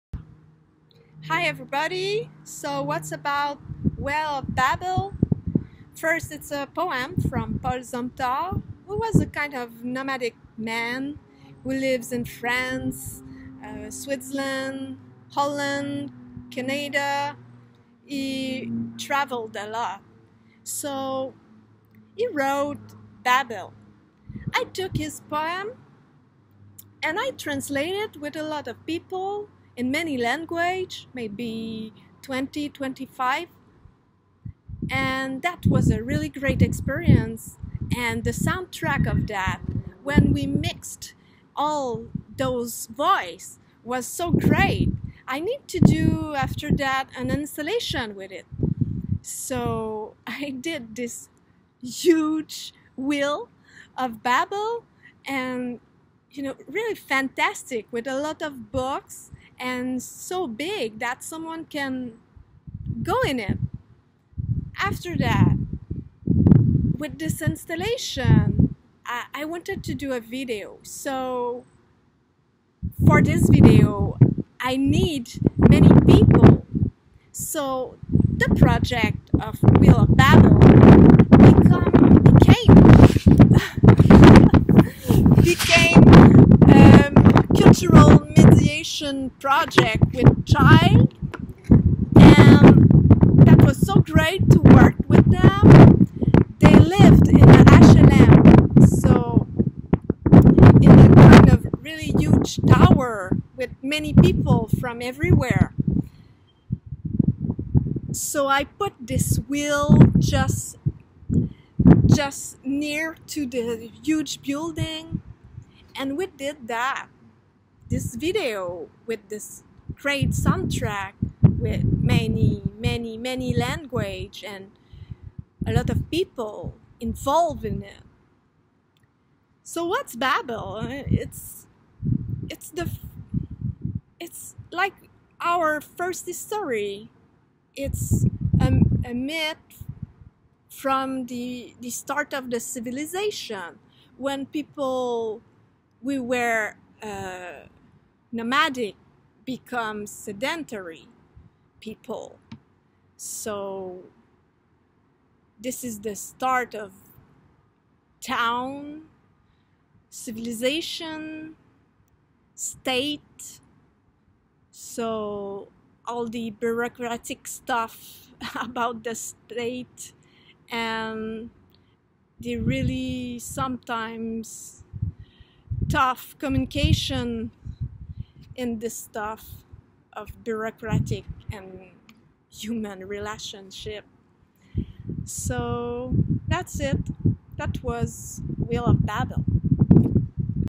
Hombres Videopoetry Award 2020_Well of Babel_Helene Matte
FINALIST - Helene Matte's interview. She tells us about the videopoetry "Well of Babel" and the theme of the Award: "The shape of the city".
(video courtesy of H. Matte)